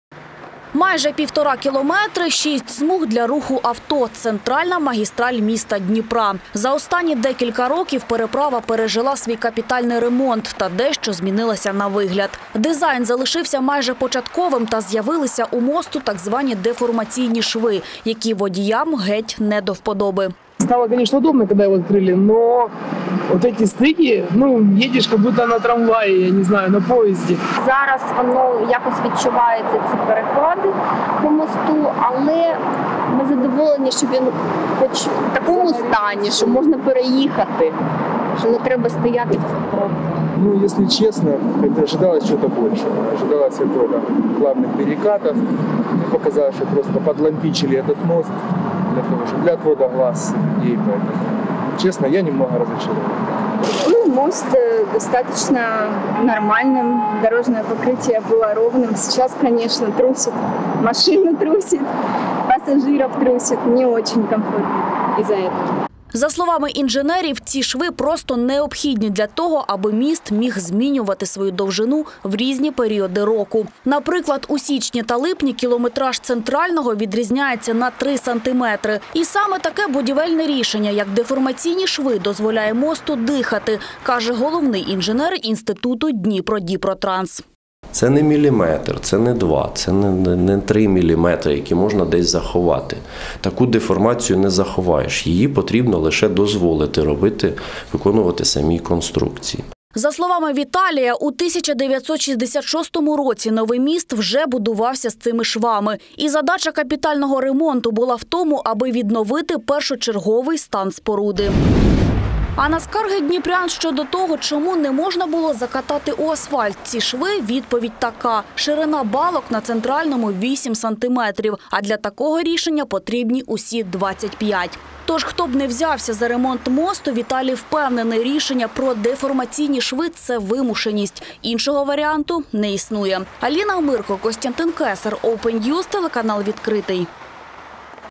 Нові подробиці щодо ремонту Центрального мосту
Нові подробиці щодо ремонту Центрального мосту. Деформаційні шви, котрі так не подобаються водіям, – це була необхідність, аби міст взагалі функціонував. Думка експертів.